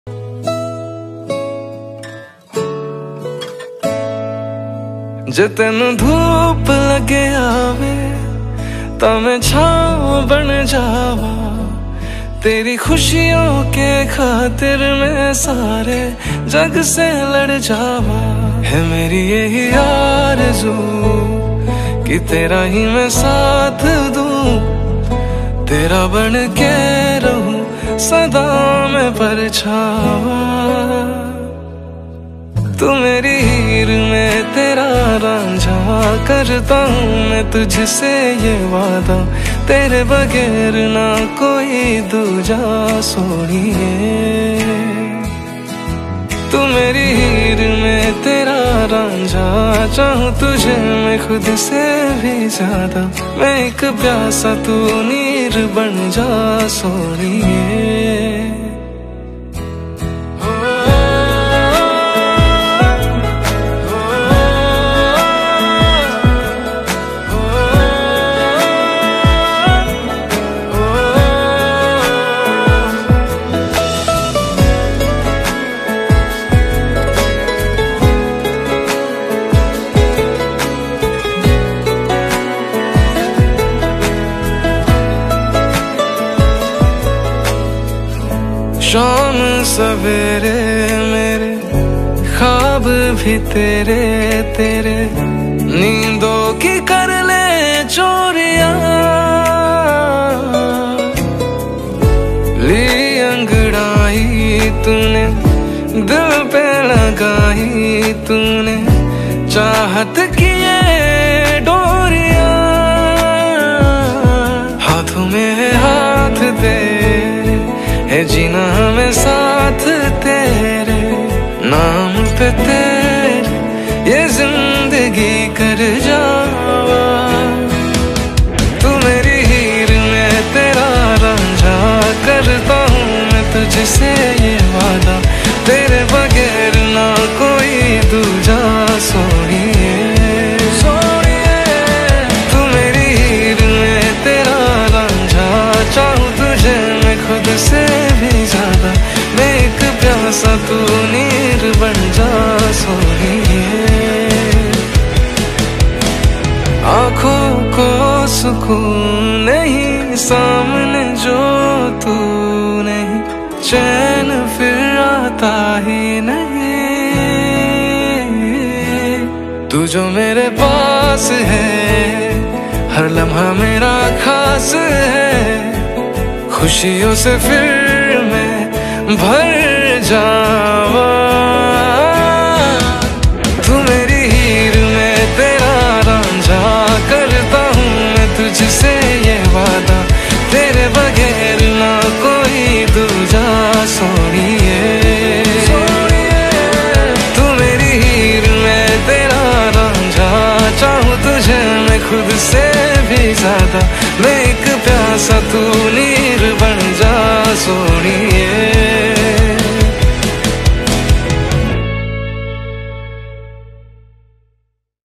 Best Pre Wedding Film 2023 | Fairmount Jaipur | The Candid House Jaipur | Fairmont Pre Wedding Film
The Candid House Photography - Changing the world of photography
As we grow older together, as we continue to change with age, there is one thing that will never change. I will always keep falling in love with you.
Met our lovely Couple: Shelly X Rahul

We love to connect & craft love stories, STOP BY AND SAY ‘HI’!